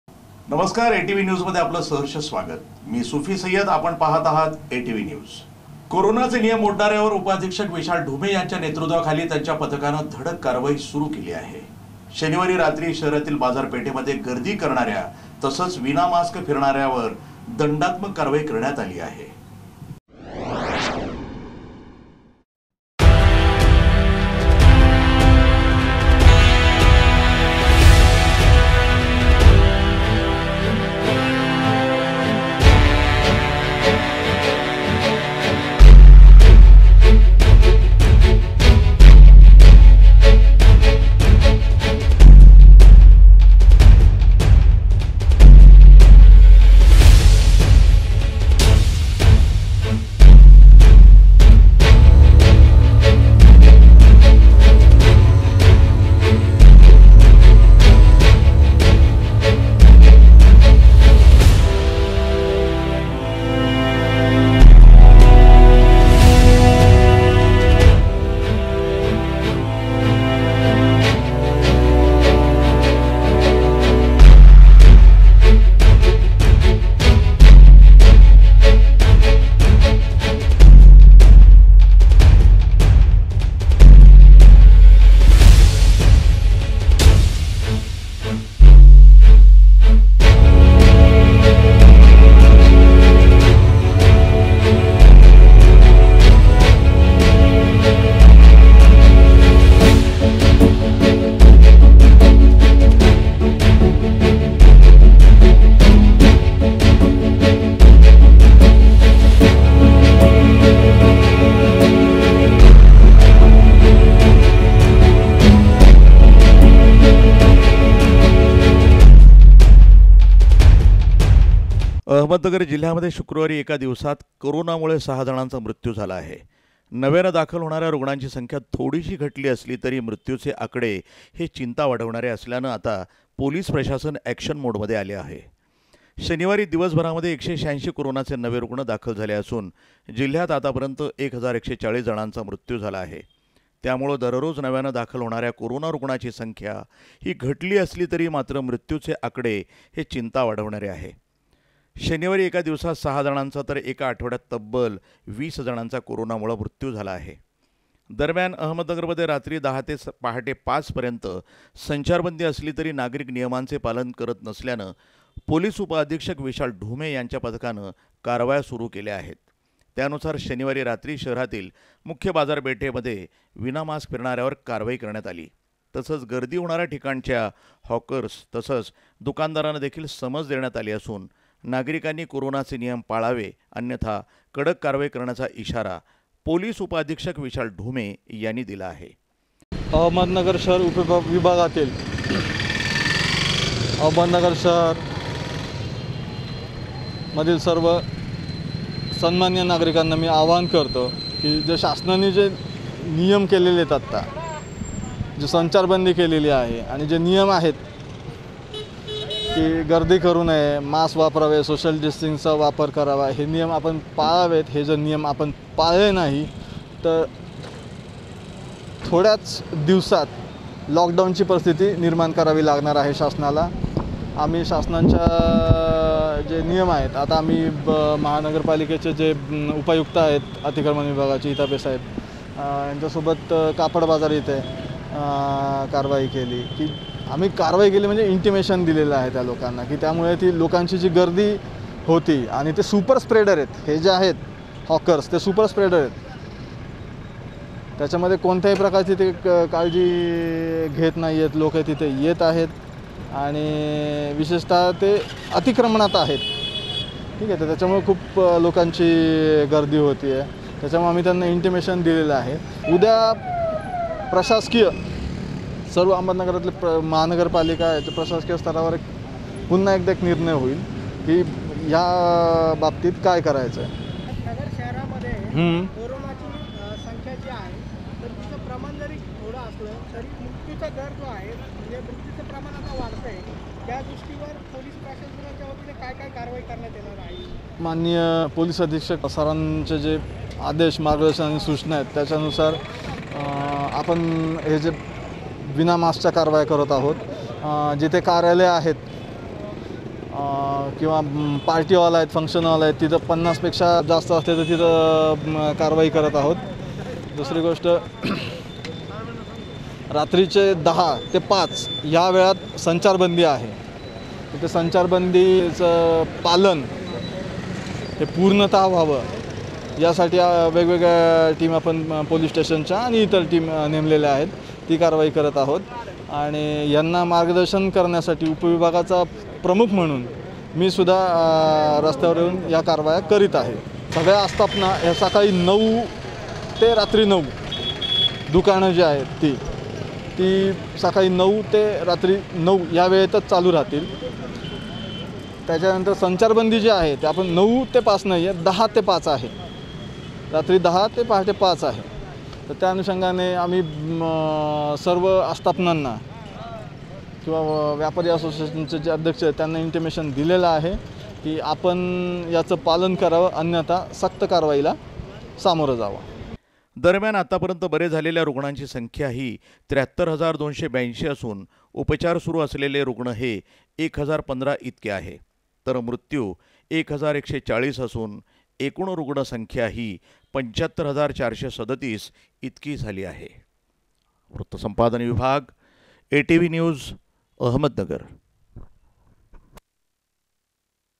कोरोनाचे नियम मोडणाऱ्यां विरोधात पोलिस प्रशासन ऍक्शन मध्ये
Latest Ahmednagar City  NEWS update कोरोनाचे नियम मोडणाऱ्यांवर उपअधीक्षक विशाल ढुमे यांच्या नेतृत्वाखाली त्यांच्या पथकाने धडक कारवाई सुरू केली आहे. शनिवारी रात्री शहरातील बाजारपेठेत गर्दी करणाऱ्या तसेच विनामास्क फिरणाऱ्यांवर दंडात्मक कारवाई करण्यात आली आहे.